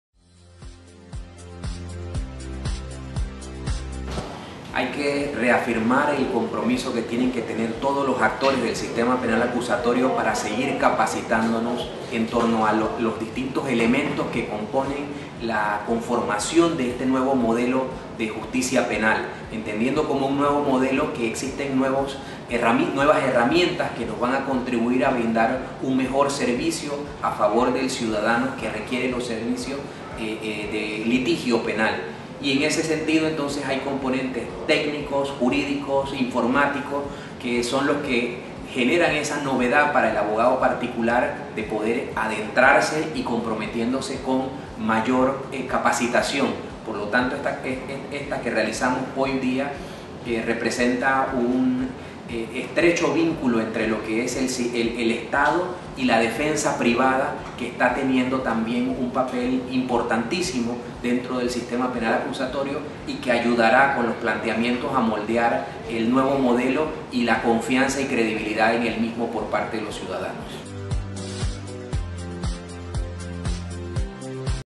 AIG capacita a Formadores del CNA en el Modulo de Gestión de Casos para Defensa Privada del SPA
La Dirección de Tecnología y Transformación gestor de la coordinación tecnológica del Sistema Penal Acusatorio (SPA),  de la Autoridad Nacional para la Innovación Gubernamental (AIG), realizó, en el Instituto de Tecnología e Innovación, el  taller “Modulo de Gestión de Casos para Defensa Privada del SPA”, dirigido a formadores, profesionales de las leyes del Colegio Nacional de Abogados de Panamá (CNA).

Con los conocimientos impartidos, los litigantes del Derecho Penal, podrán gestionar sus expedientes electrónicos en cualquier parte del mundo, así como ejecutar consultas de otros casos y recibir notificaciones de audiencias; además de hacer solicitudes a las oficinas judiciales en torno a: audiencias, medidas cautelares, fianzas, interponer recursos; entre otras actuaciones incluidas en la Ley 63 de Agosto del 2008 (Código Procesal Penal de Panamá), explicó la licenciada Ana Castro, abogada de AIG en el proyecto Sistema Penal Acusatorio.

 Juan Carlos Araúz, Vicepresidente del CNA, señaló que “Hay que reafirmar el compromiso que tienen todos los actores del SPA, para seguir capacitándonos en torno a los distintos elementos que componen la conformación de este nuevo modelo de Justicia Penal, entendiendo que existen nuevas herramientas que contribuirán a brindar un mejor servicio de litigio penal  a favor del ciudadano que los requiera”.

Con estas capacitaciones se espera que el Colegio Nacional de Abogados se convierta en un ente replicador de otros gremios del Derecho adscritos a su membresía y que promuevan el uso de los medios digitales, haciendo referencia a la Ley 75 de Diciembre de 2015, que dicta medidas sobre la formación de los expedientes electrónicos (carpetillas), en concordancia con la Ley 83 de 2012, que establece los procesos de trámites gubernamentales vía web, parte del programa Panamá en Línea.